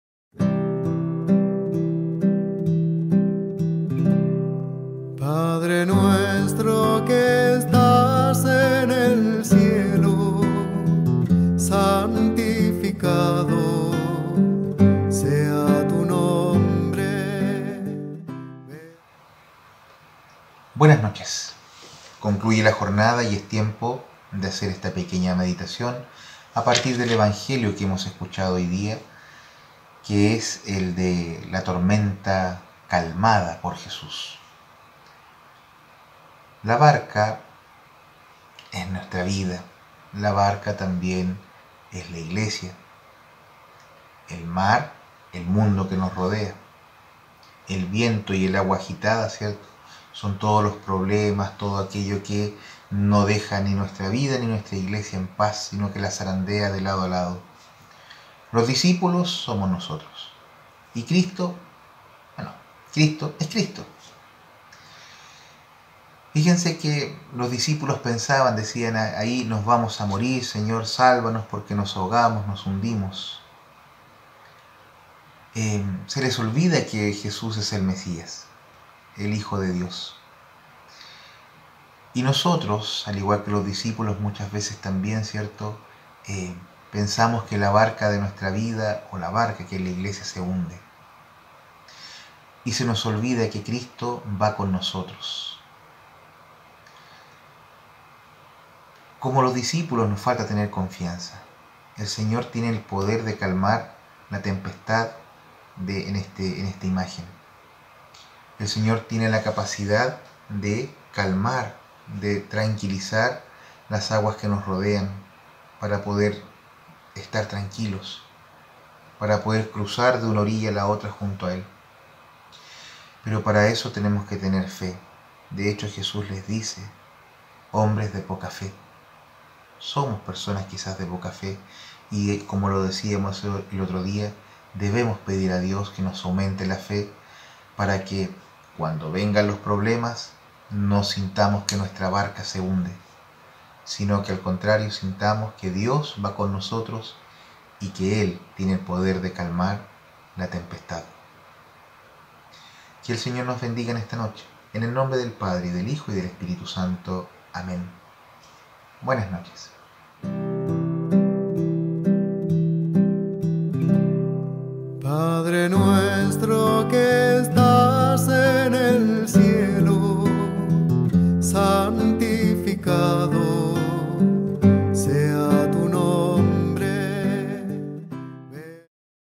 Mensaje de Buenas Noches Martes 4 de Julio
Reflexión del Padre César Pedreros